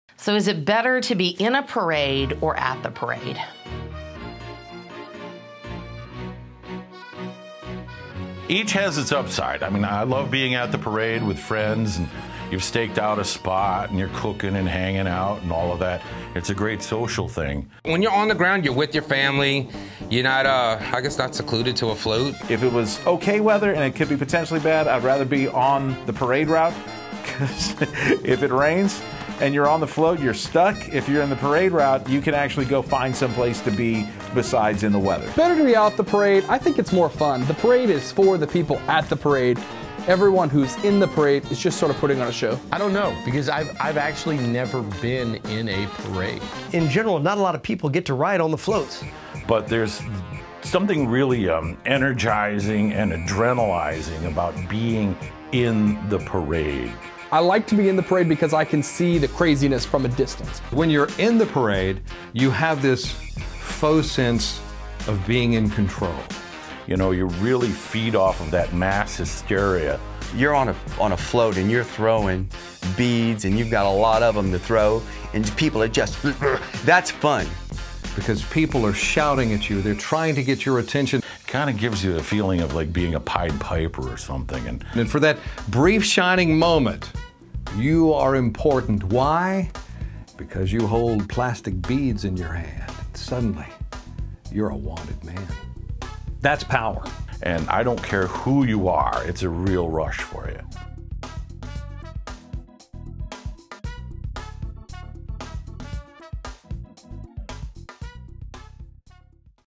In The Parade Vs At The Parade | Mardi Gras 101
Is it better to be in the parade or at the parade? Our panel of Mardi Gras experts discuss.